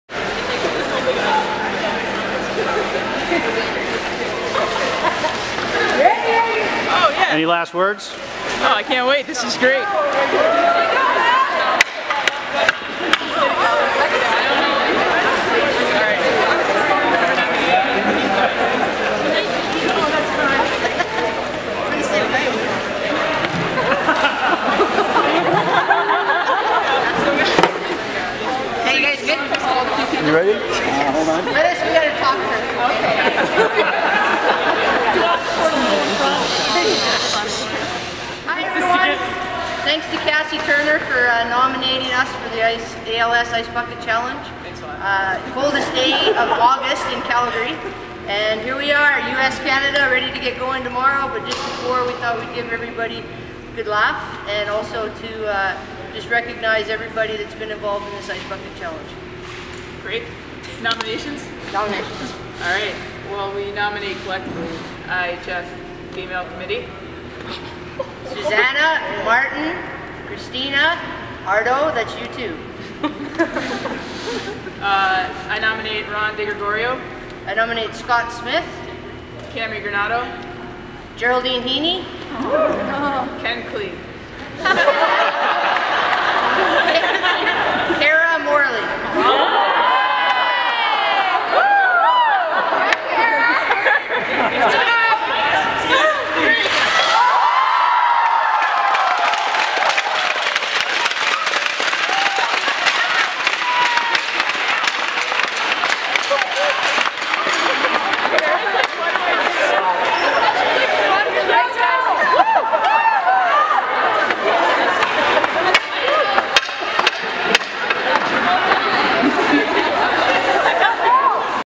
ALS Ice Bucket Challenge - 2014 USA Women's Hockey
Check out this behind the scenes look at the collaborative effort between the GM's of the U.S. and Canada's women's hockey teams as they take part in the ALS Ice Bucket Challenge.